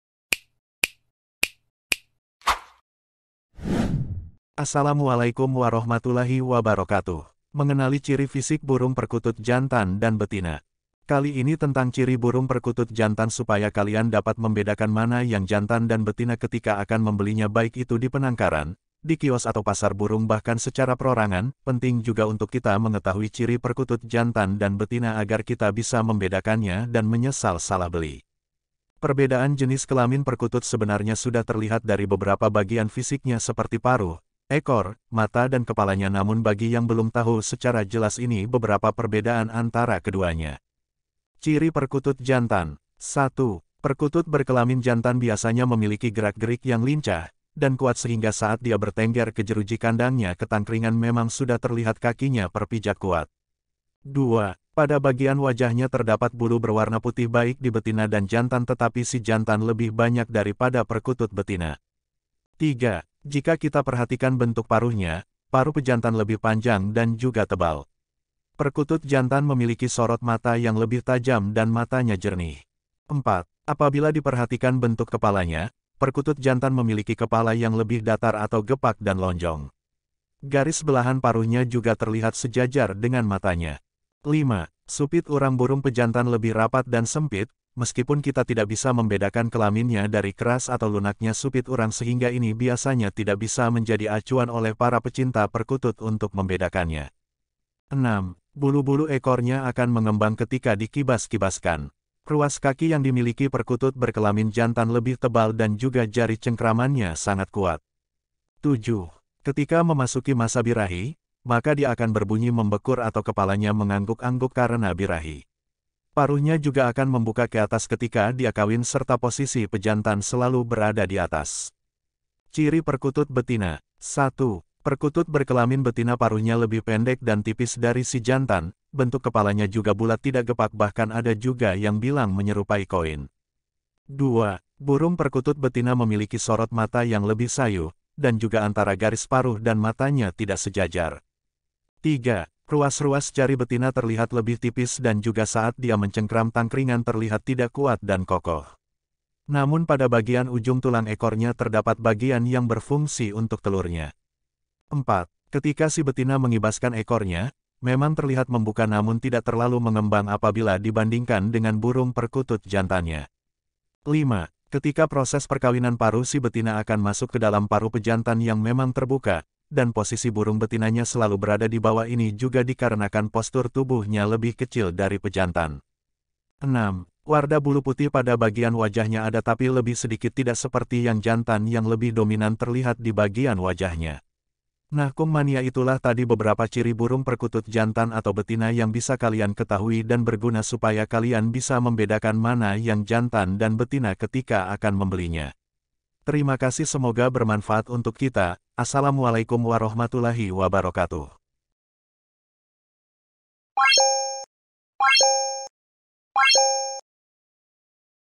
Ciri-ciri Perkutut Jantan Dan Betina
Mengenali Ciri Fisik Burung Perkutut Jantan Dan Betina.
Kali ini tentang Ciri Burung Perkutut Jantan supaya kalian dapat membedakan mana yang jantan dan betina ketika akan membelinya baik itu dipenangkaran, di kios atau pasar burung bahkan secara perorangan.penting juga untuk kita mengetahui ciri perkutut jantan dan betina agar kita bisa membedakannya dan menyesal salah beli.

Perbedaan jenis kelamin perkutut sebenarnya sudah terlihat dari beberapa bagian fisiknya seperti paruh, ekor, mata dan kepalanya namun bagi yang belum tahu secara jelas ini beberapa perbedaan antara keduanya.

Ciri Perkutut Jantan
1. Perkutut berkelamin jantan biasanya memiliki gerak gerik yang lincah, dan kuat sehingga saat dia bertengger ke jeruji kandangnya ke tangkringan memang sudah terlihat kakinya perpijak kuat.

2. Pada bagian wajahnya terdapat bulu berwarna putih baik di betina dan jantan tetapi si jantan lebih banyak dari pada perkutut betina.

3. Jika kita perhatikan bentuk paruhnya, paruh pejantan lebih panjang dan juga tebal. Perkutut jantan memiliki sorot mata yang lebih tajam dan matanya jernih.

4. Apabila diperhatikan bentuk kepalanya, perkutut jantan memiliki kepala yang lebih datar atau gepak dan lonjong. Garis belahan paruhnya juga terlihat sejajar dengan matanya.

5. Supit urang burung pejantan lebih rapat dan sempit, meskipun kita tidak bisa membedakan kelaminnya dari keras atau lunaknya supit urang sehingga ini biasanya tidak bisa menjadi acuan oleh para pecinta perkutut untuk membedakannya.

6. Bulu-bulu ekornya akan mengembang ketika dikibas-kibaskan. Ruas kaki yang dimiliki perkutut berkelamin jantan lebih tebal dan juga jari cengkramannya sangat kuat.

7. Ketika memasuki masa birahi, maka dia akan berbunyi membekur atau kepalanya mengangguk-angguk karena birahi. Paruhnya juga akan membuka keatas ketika dia kawin serta posisi pejantan selalu berada diatas.
Ciri Perkutut Betina
1. Perkutut berkelamin betina paruhnya lebih pendek dan tipis dari si jantan. bentuk kepalanya juga bulat tidak gepak bahkan ada juga yang bilang menyerupai koin.

2. Burung perkutut betina memiliki sorot mata yang lebih sayu, dan juga antara garis paruh dan matanya tidak sejajar.

3. Ruas-ruas jari betina terlihat lebih tipis dan juga saat dia mencengkram tangkringan terlihat tidak kuat dan kokoh. Namun pada bagian ujung tulang ekornya terdapat bagian yang berfungsi untuk telurnya.

4. Ketika si betina mengibaskan ekornya, memang terlihat membuka namun tidak terlalu mengembang apabila dibandingkan dengan burung perkutut jantannya.

5. Ketika proses perkawinan paruh si betina akan masuk kedalam paruh pejantan yang memang terbuka, dan posisi burung betinanya selalu berada dibawah ini juga dikarenakan postur tubuh nya lebih kecil dari pejantan.

6. Warda bulu putih pada bagian wajahnya ada tapi lebih sedikit tidak seperti yang jantan yang lebih dominan terlihat dibagian wajahnya.

nah kung mania itulah tadi beberapa Ciri Burung Perkutut Jantan atau betina yang bisa kalian ketahui dan berguna supaya kalian bisa membedakan mana yang jantan dan betina ketika akan membelinya.
terimakasih semoga bermanfaat untuk kita. assalamualaikum Warohmatullahi Wabarokatuh.